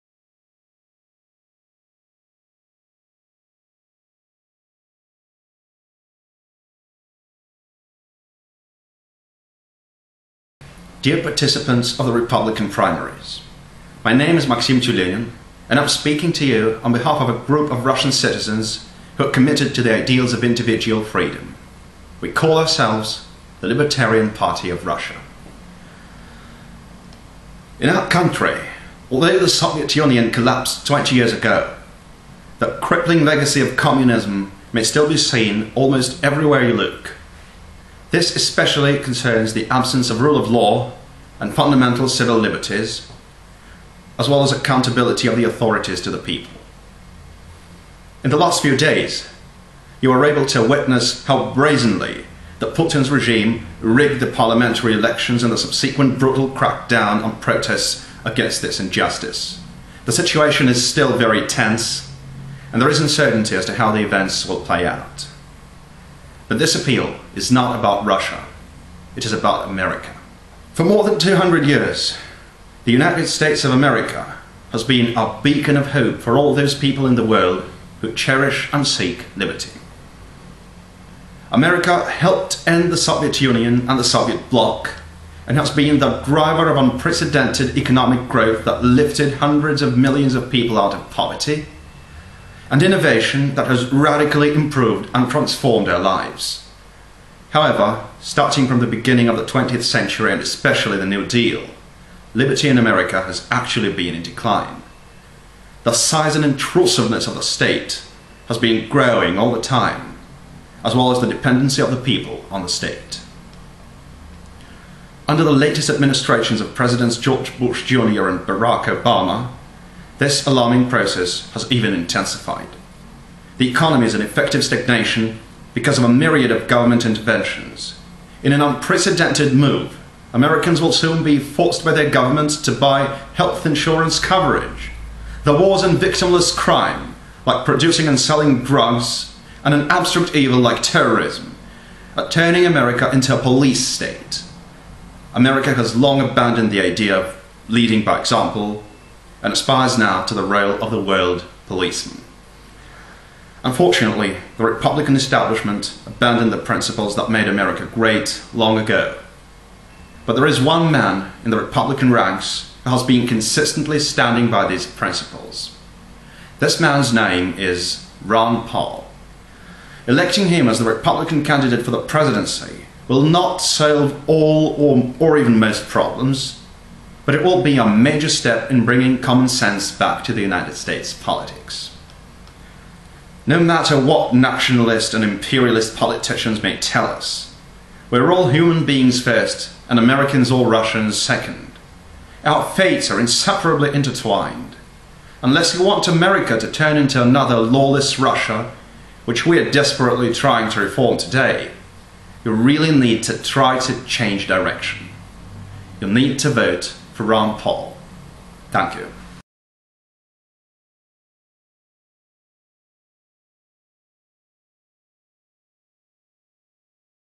Appeal to Republicans from Libertarian Party of Russia
Unfortunately, the Republican establishment abandoned the principles that made
America great long ago. But there is one man in the Republican ranks who has
been consistently standing by these principles. This man is Ron Paul. Electing
him as the Republican candidate for the presidency will not solve all or even most
problems. But it will be a major step in bringing common sense back to the U.S.
politics.
No matter, what nationalist and imperialist politicians may tell us, we are all
human beings first and Americans and Russians second. Our fates are inseparably
intertwined. Unless you want America to turn into another lawless Russia which
we are desperately trying to reform today, you really need to try to change
direction. You need to vote for Ron Paul. Thank you.